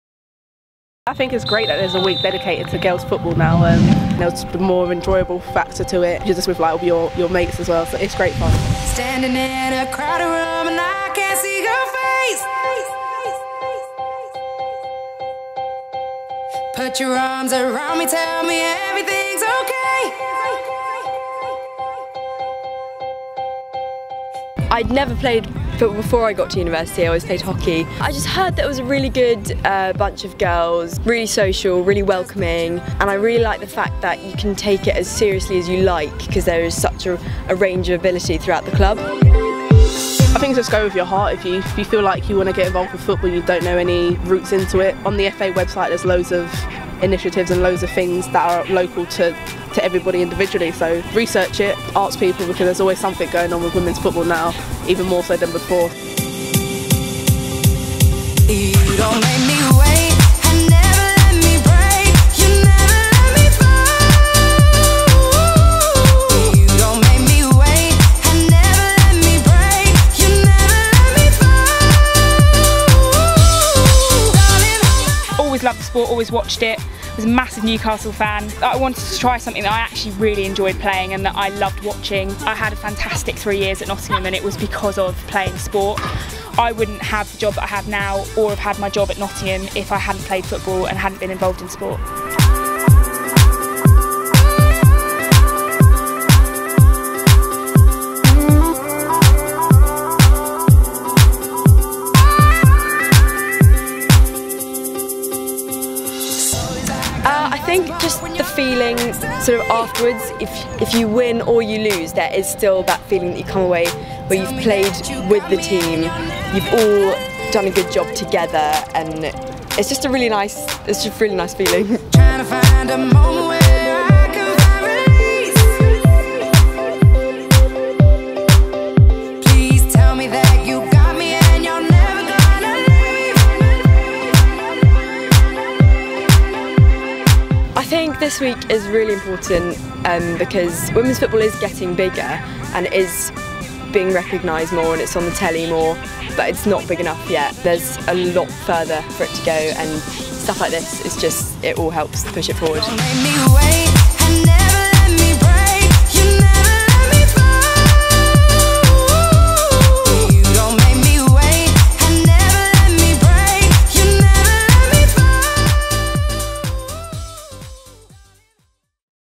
WeCanPlay Episode I
BUCS Football Development are proud to launch it's new female specific series ‪#‎WeCanPlay‬ which will highlight some fantastic role models within Football and Futsal. Over the course of the year we will be releasing videos highlighting different aspects of the football offer in Higher Education.

Episode One goes back to October when Girls' Football Week was delivered and tells the story of Fleur Noble of Royal Holloway Sport, Danielle Carter England and Arsenal Ladies Striker and BUCS Intern Kiri Madhani, and why they play and value football.